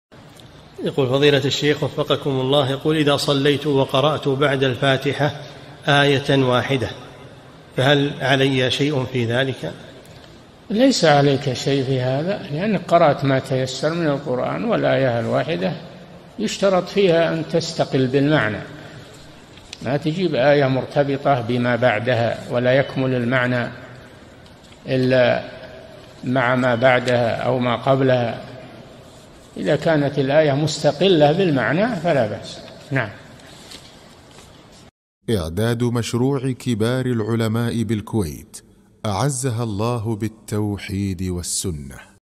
[452 -582] من قرأ آية واحدة بعد الفاتحة وركع، هل عليه شيء؟ - الشيخ صالح الفوزان
-"السؤال: يقول: إذا صليت وقرأت بعد الفاتحة آية واحدة فهل عليّ شيء في ذلك؟
الجَوَابُ: ليس عليك شيء في هذا؛ لأنك قرأت ما تيسر من القرآن، والآية الواحدة يشترط فيها أن تستقل بالمعنى ما تجيء بآية مرتبطة بما بعدها ولا يكمل المعنى إلا مع ما بعدها أو ما قبلها إذا كانت الآية مستقلة بالمعنى فلا بأس.
للاشتراك في قناة الشيخ صالح الفوزان
سؤال وجواب من شرح صفة الصلاة ( 582 ) مقطع

للاستفسار والإقتراحات والدعم
رئيس الفريق
لمشروع كبار العلماء